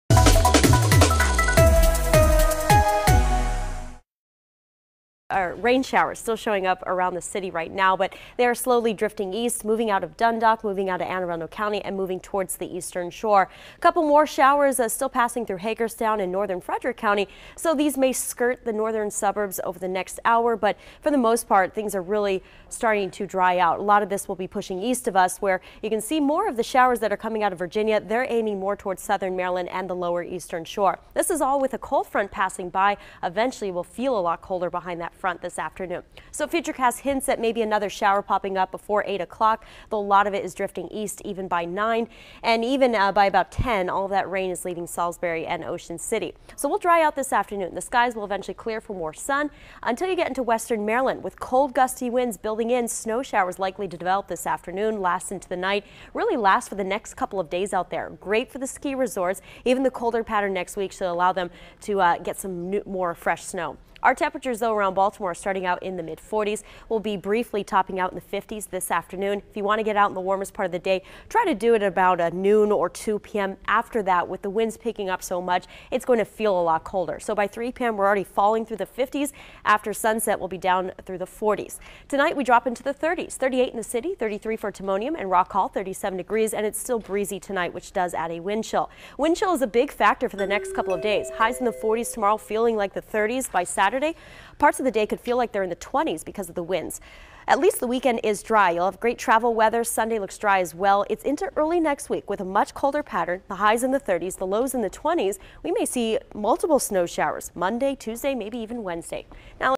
Temps to reach 50s Thursday
During the Thursday morning weather forecast, Ava says we can expect temperatures to reach the 50s.